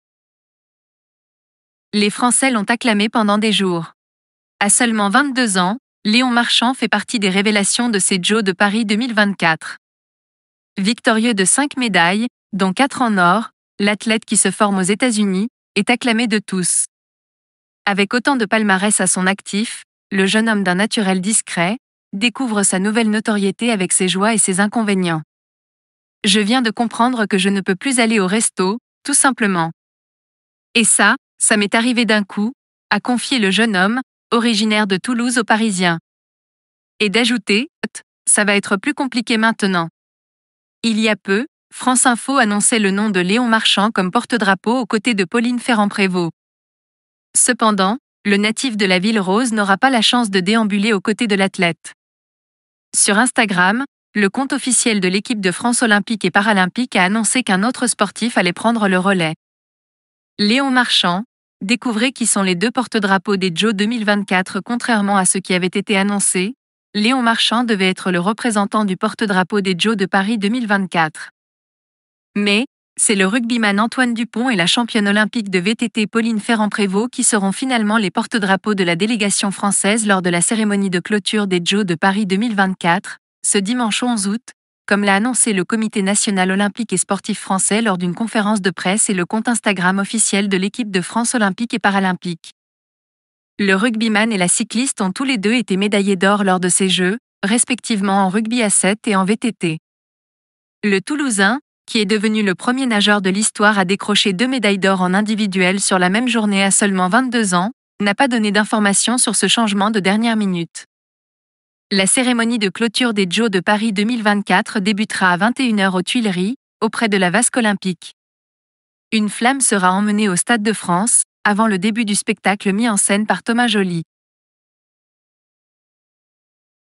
JO de Paris 2024 : Léon Marchand n'est finalement plus le porte-drapeau pour la clôture ?!
Décidément, Léa Salamé n'en finit plus de faire parler d'elle... Confrontée à diverses polémiques ces derniers jours, la journaliste se retrouve depuis plusieurs heures face à un gros lapsus qu'elle a fait en pleine interview du journaliste sportif Philippe Lafon.